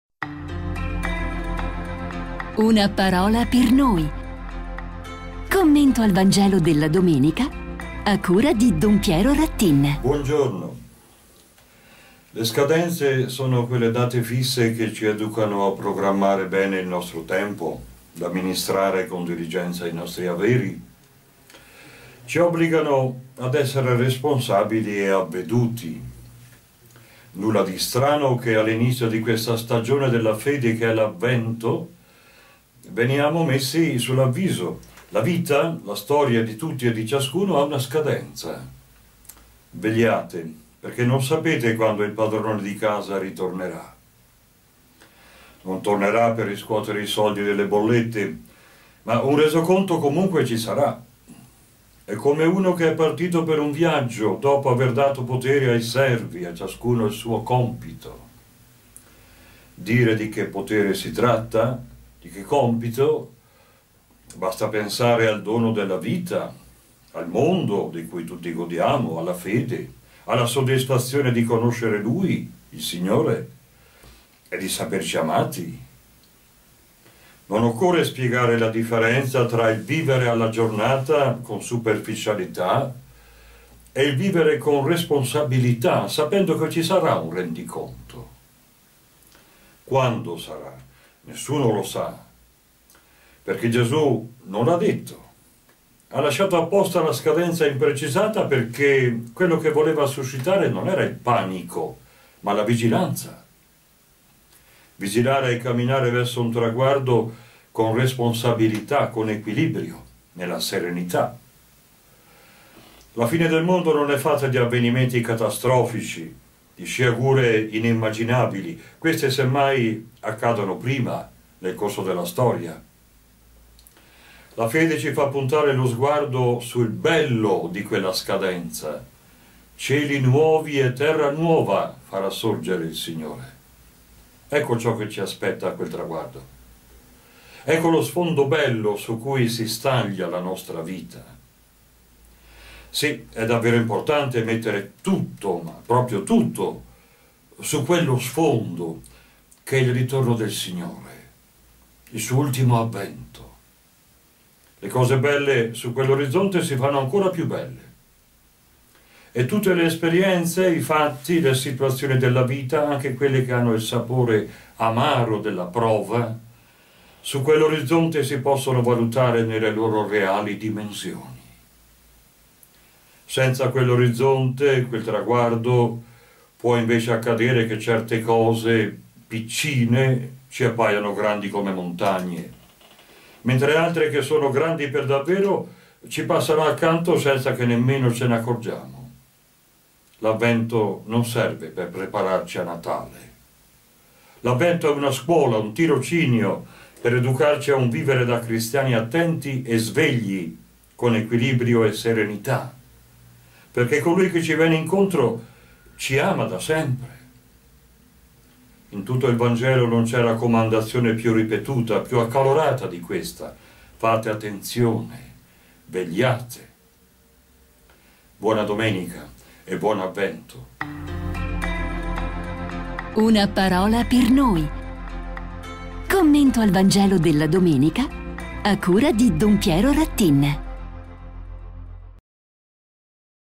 Una Parola per noi - domenica 30 novembre (I Avvento)
Vivere non è andare a zonzo
Anche il Signore, nel vangelo della prossima domenica e all’inizio di questa stagione della Fede che è l’Avvento, ci parla di una scadenza. La mia vita, la storia di tutti e di ciascuno, ha una scadenza: “Vegliate, perchè non sapete quando il padrone di casa

I lettura: Isaia 63,16b -17.19b;
II lettura: 64,2-7; 1Corinzi 1,3-9;
Vangelo: Marco 13,33-37